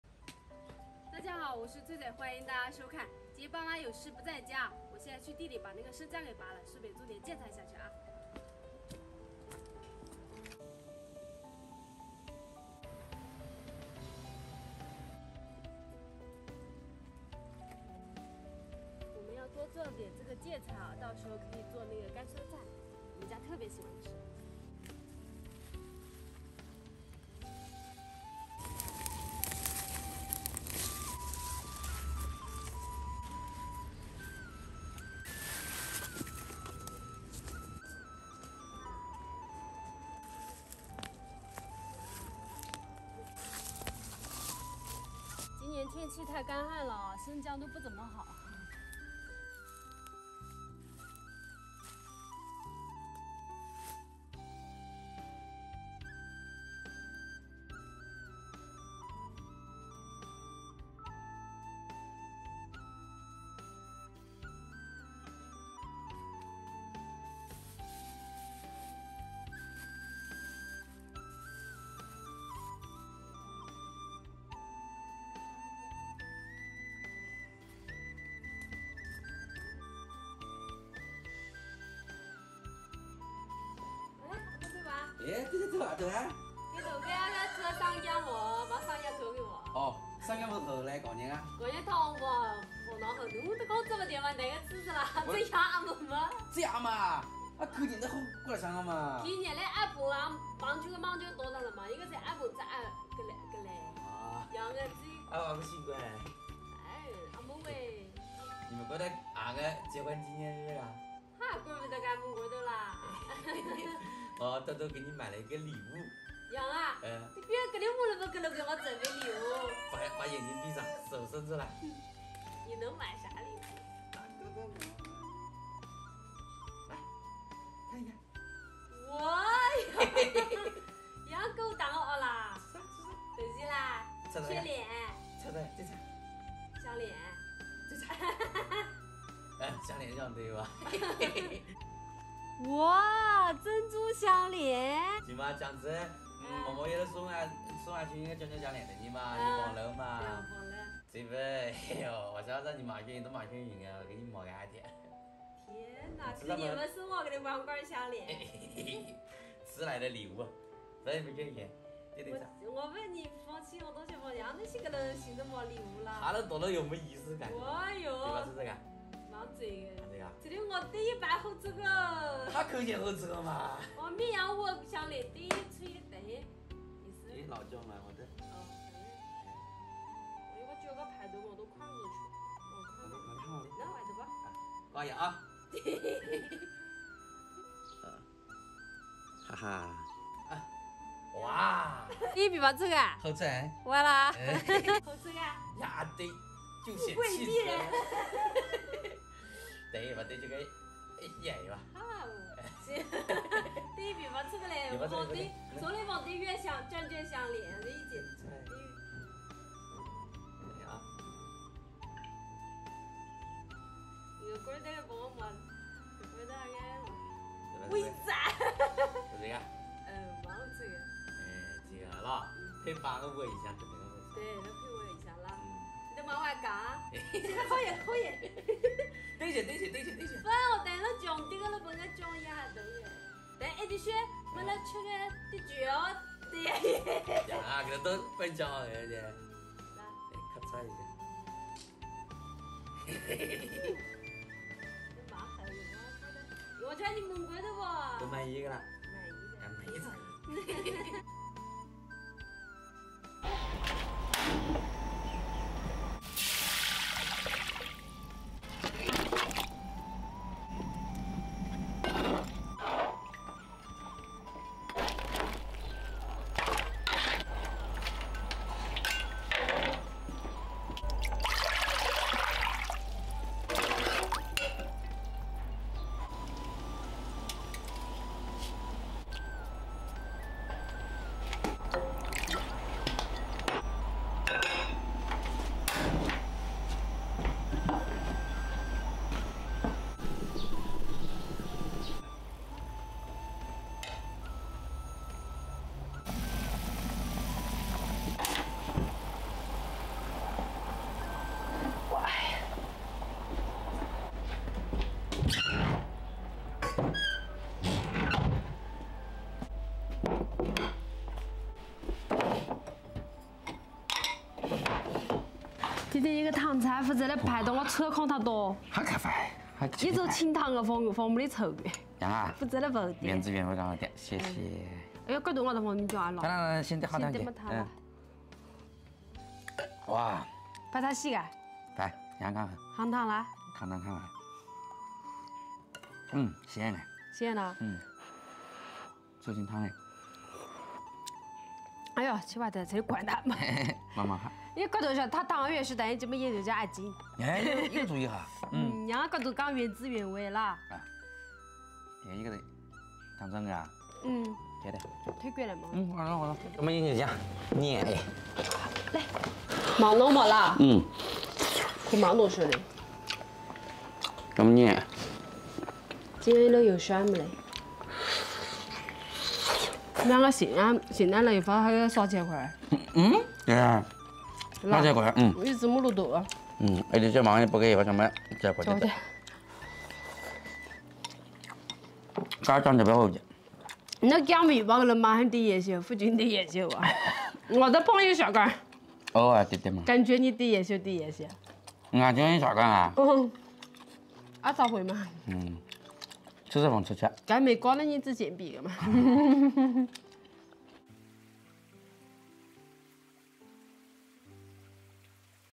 结婚纪念日过了一个多月，老公偷偷给媳妇准备啥礼物？媳妇感动了
结婚纪念日过了一个多月，农村老公偷偷给媳妇准备啥礼物？媳妇感动了